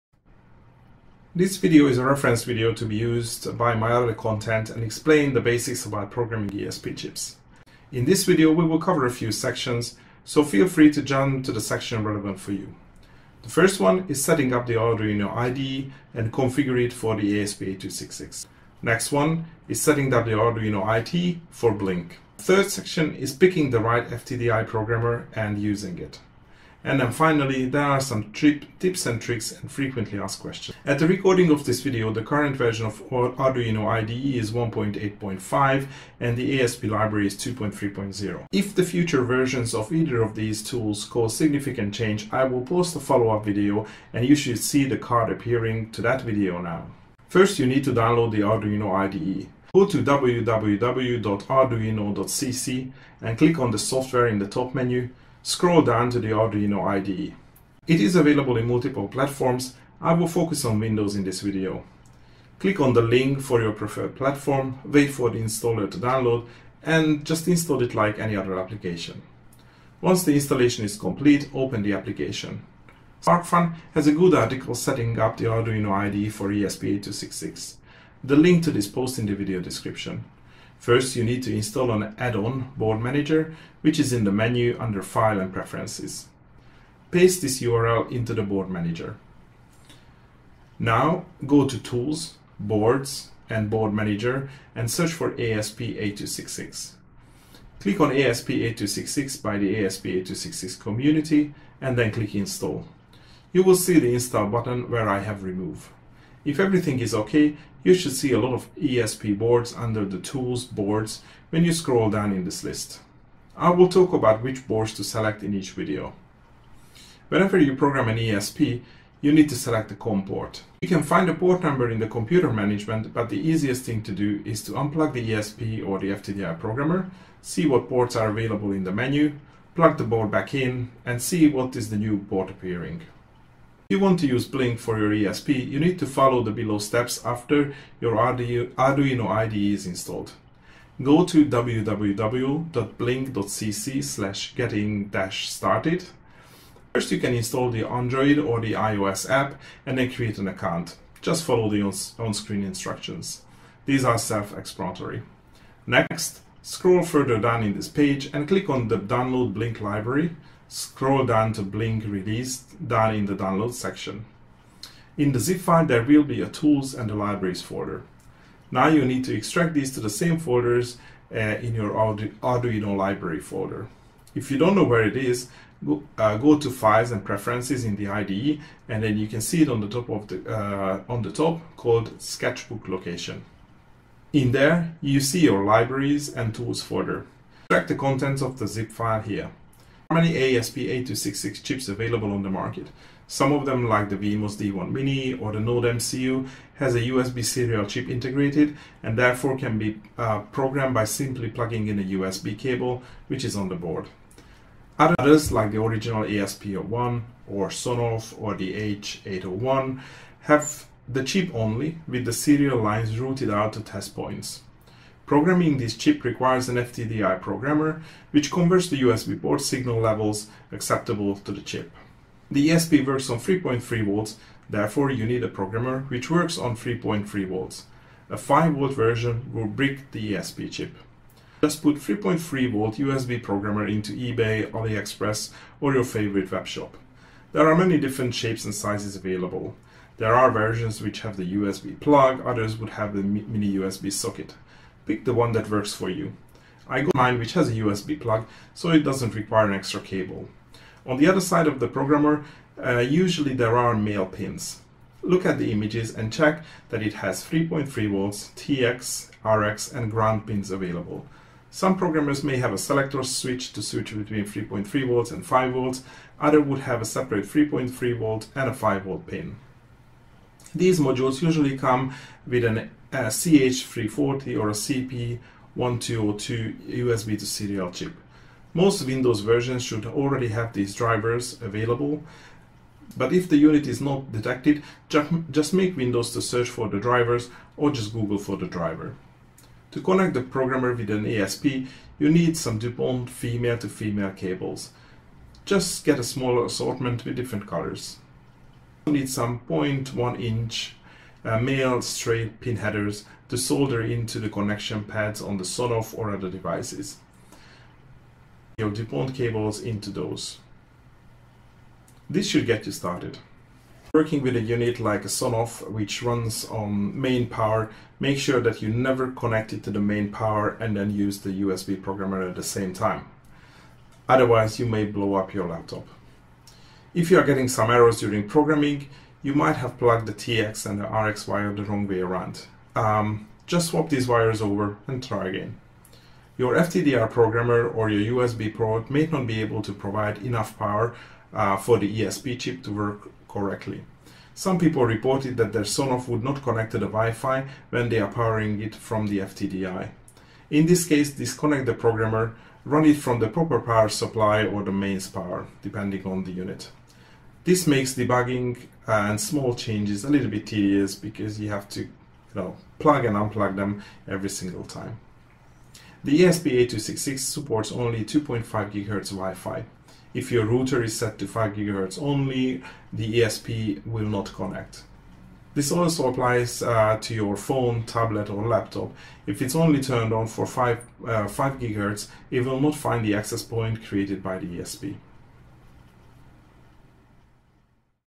ESP Programming Basics
This video will be used by my other content to explain the basics to program ESP 8266 chips.

Article mentioned in the video for setting up Arduino IDE for ESP 8266: